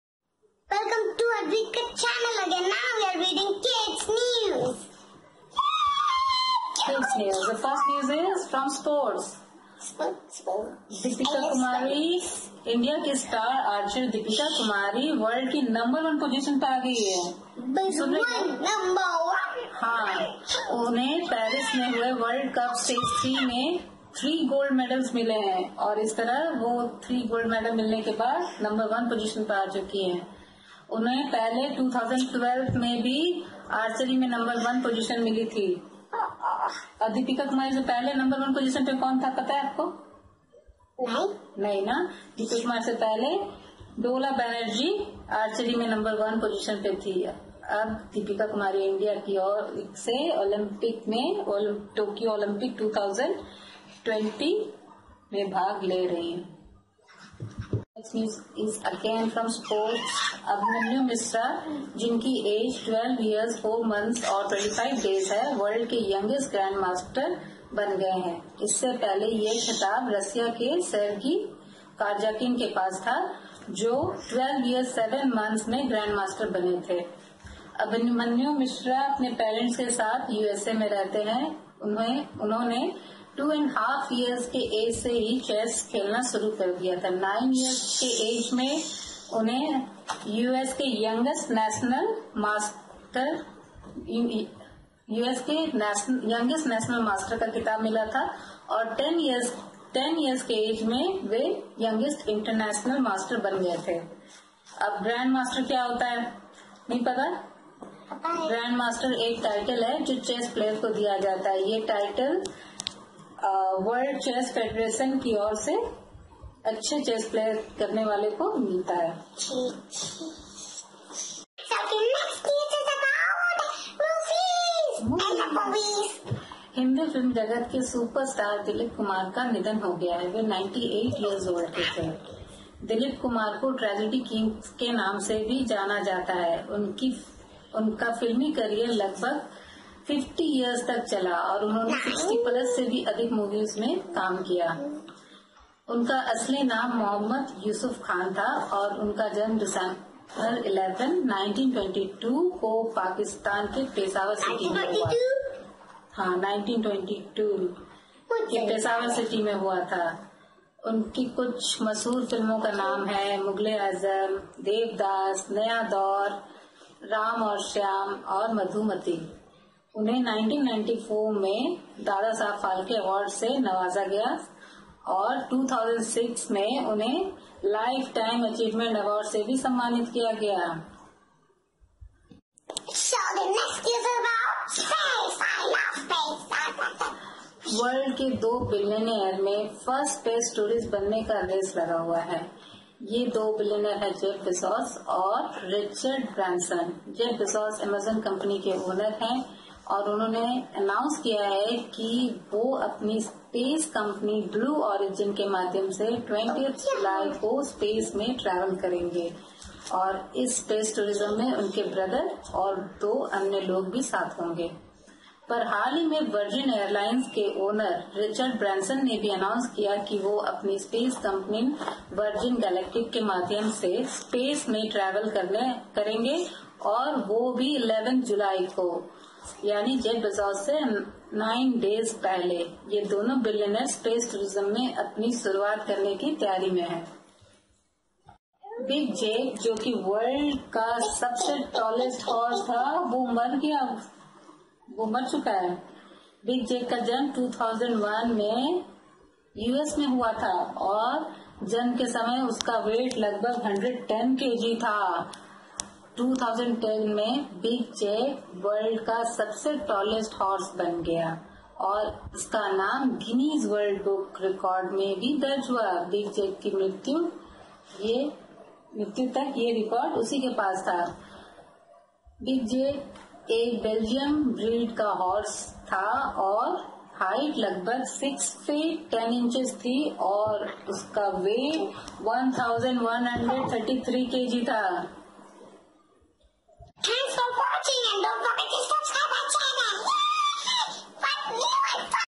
Kids News Week July 2021 | World Top Archer | Youngest GM | Dilip Kumar | Tallest Horse | Space Tour
Advika and her mumma have come up with latest news from the field of Sports, Space, Cinema and World. The news covered in this video are as follows-
0:10 Deepika Kumari regain top position in archery
0:50 Abhimanyu Mishra becomes youngest GM
2:05 Tragedy King Dilip Kumar dies at 98
3:08 Jeff bezos and Richard Branson in race to become first space tourist
4:00 World Tallest Horse